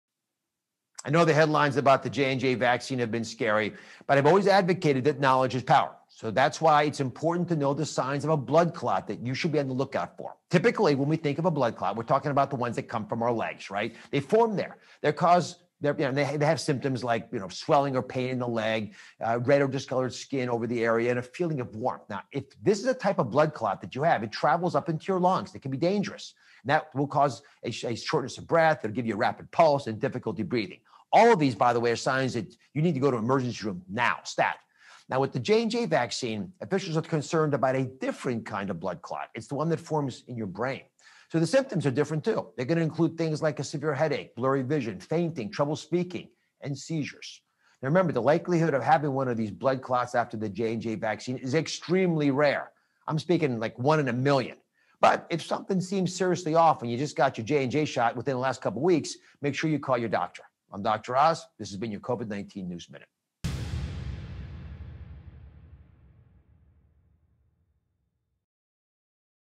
Daily Covid-19 Minute: Signs of A Blood Clot
The headlines about the Johnson & Johnson vaccine have been everywhere so it’s important to know the signs of a blood clot you should be on the lookout for. Typically, when we think of blood clots, we’re talking about the ones that come from our legs, they form there.  They have symptoms like swelling or pain in your leg, red or discolored skin over the area, and a feeling of warmth. With this type of blood clot, it travels up into your lungs and can be dangerous. It will cause shortness of breath, rapid pulse, and difficulty breathing. All of these are signs you need to go to an emergency room immediately.

With the Johnson & Johnson vaccine, officials are concerned about a different kind of blood clot. This clot forms in your brain so the symptoms are different too. These symptoms include severe headache, blurry vision, fainting, trouble speaking, and seizures. Remember, the likelihood of having one of these blood clots after the Johnson & Johnson vaccine is extremely rare, like one in a million, but if something seems seriously off and you just received your Johnson & Johnson shot within the last couple of weeks, make sure to call your doctor.